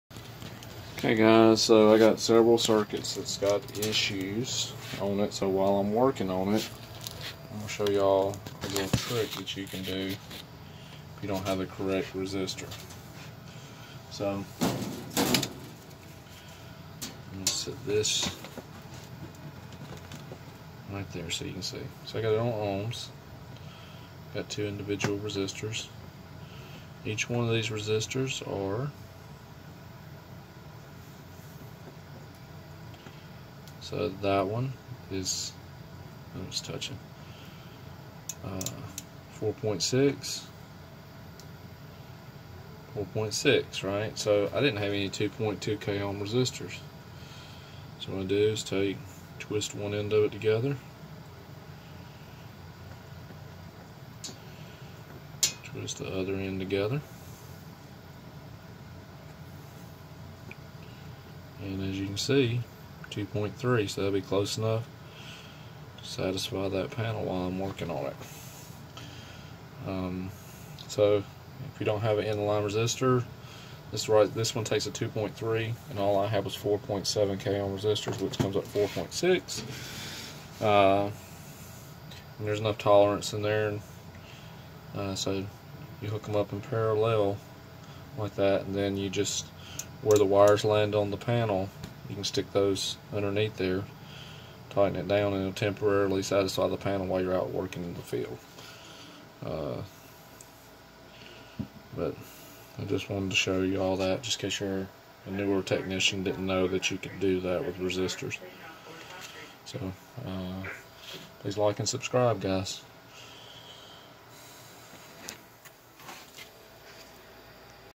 Eol oh no I do not have a 2.2 k ohm resistor what do I do? End of line resistor for nac panel.
Eol oh no I do not have a 2.2 k ohm resistor what do I do?